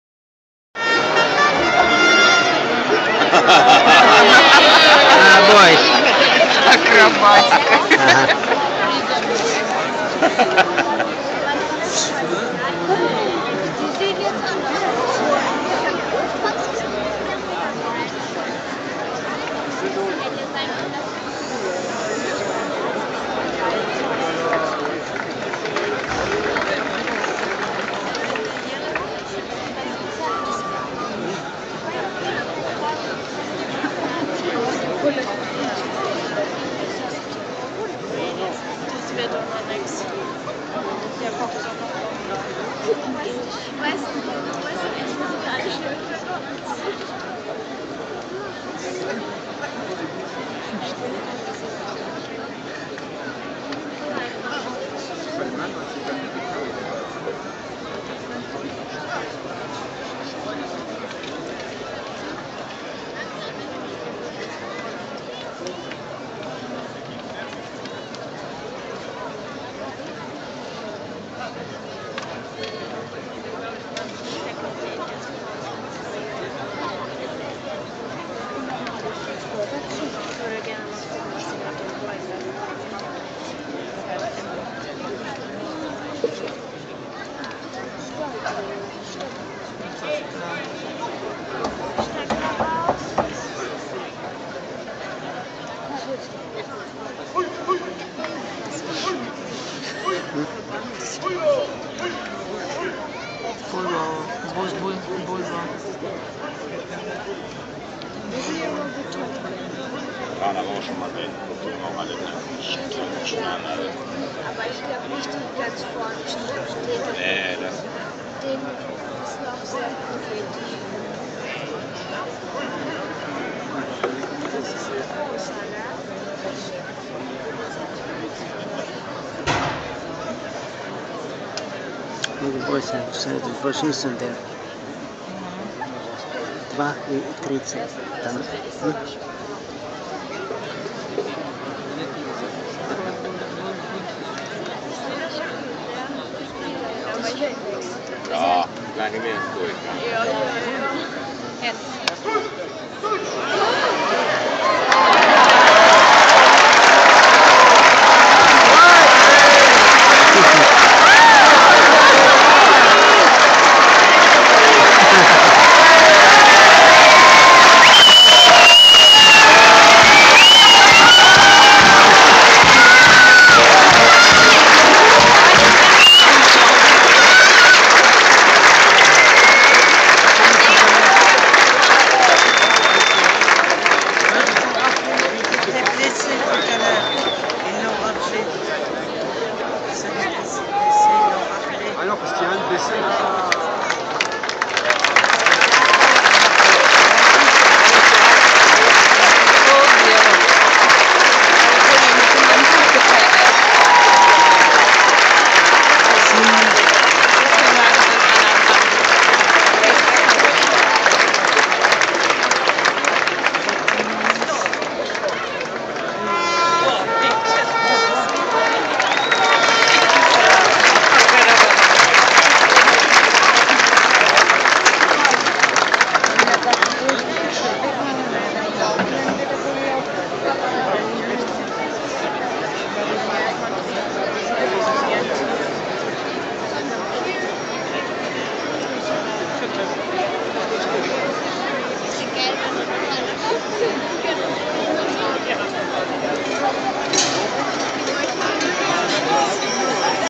Bullfighting in Portugal. Albufeira - 2011
For wounded bulls provide an opportunity to take revenge on people. Sometimes they get it!